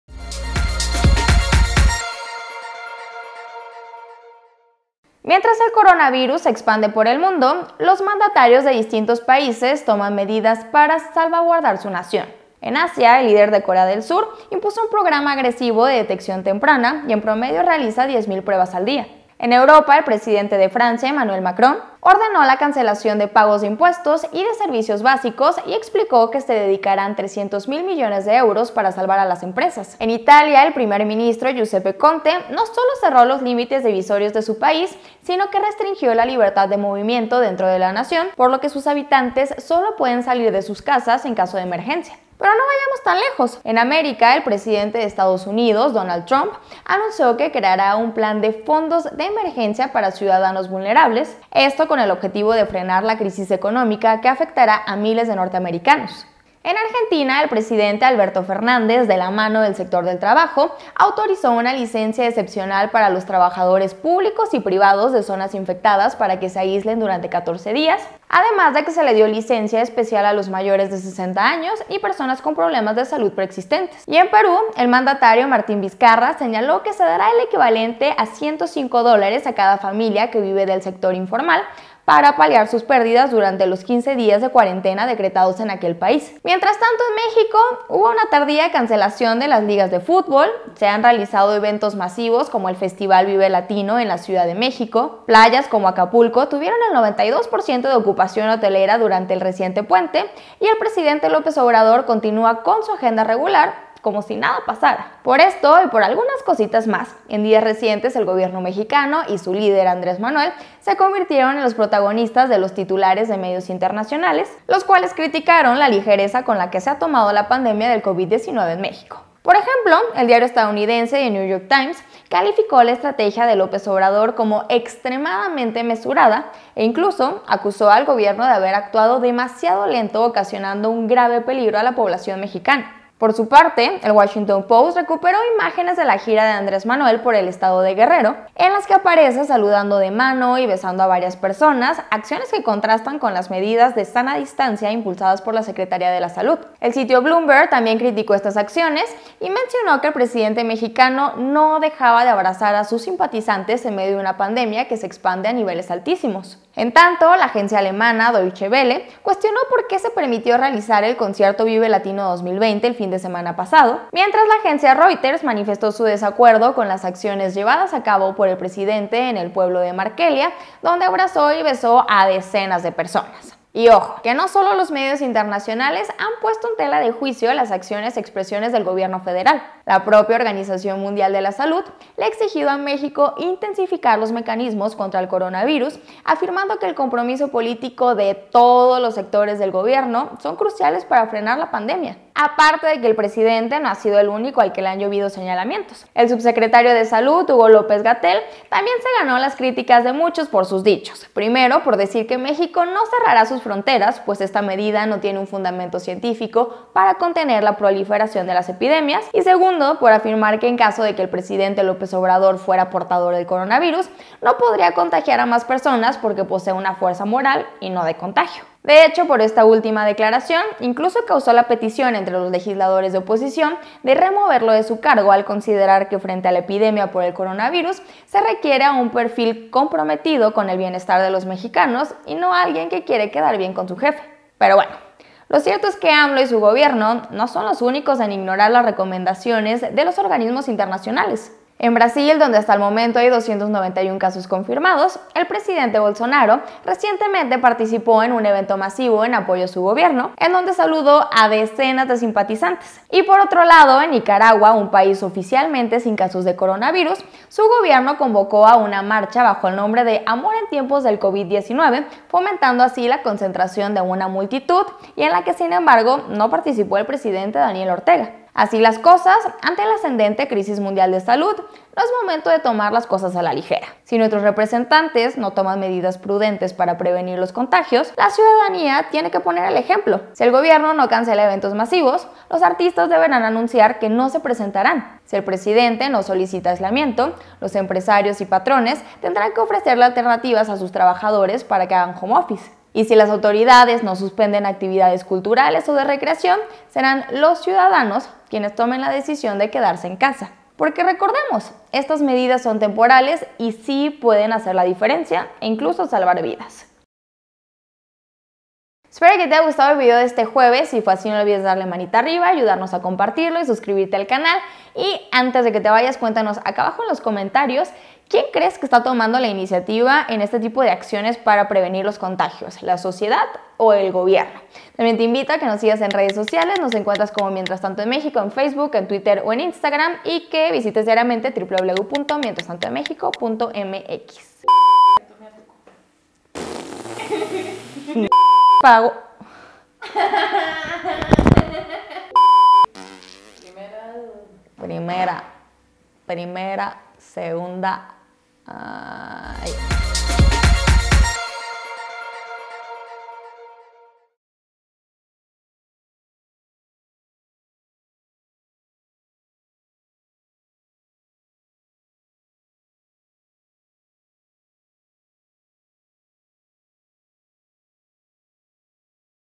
Medios internacionales critican la ligereza de AMLO ante el coronavirus | Mientras Tanto en México
Debido a la ligereza con la que el Presidente Andrés Manuel López Obrador ha tomado la pandemia del coronavirus, a México le llovieron varias críticas y burlas en todo el mundo.